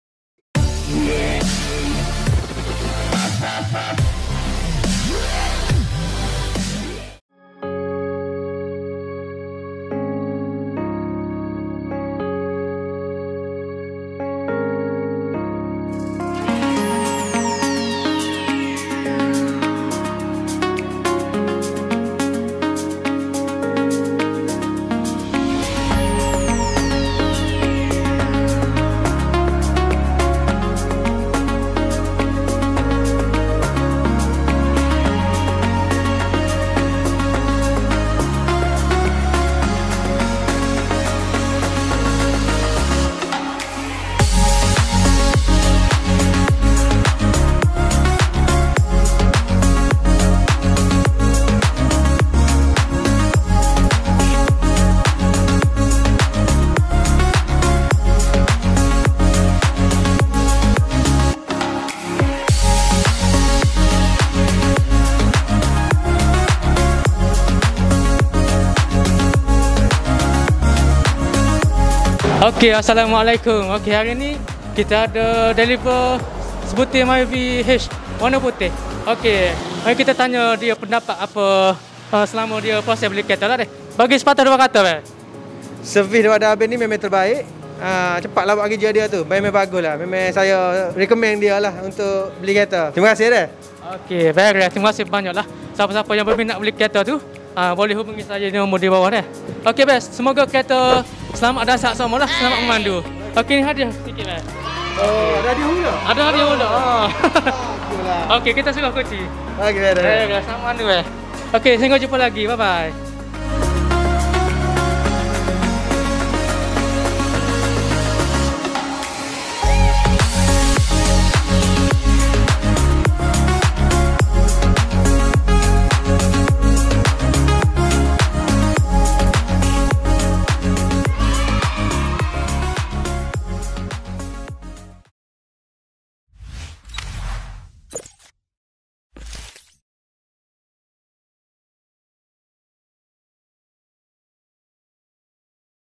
DELIVER | PERODUA MYVI H
🎀Alhamdulillah [ Delivered]🎀
Tahniah & Syabas Semua customer y telah dpt kereta..selesai penyerahan register bulan 1. Terima kasih xterhingga ats kesudian berurusan dgn sy & sabar mnunggu kreta.
Alhamdulillah, urusan dipermudahkan Oleh Allah dari awal smpi dapat kreta.

Didoakan semoga Kereta Dlm keadaan baik, Selamat & Sihat Sentiasa, Mampu berkhidmat ke pergi kemana2..🎁✨
Untuk y berminat nk booking, Jgn malu2 jgn segan2 utk wasap sy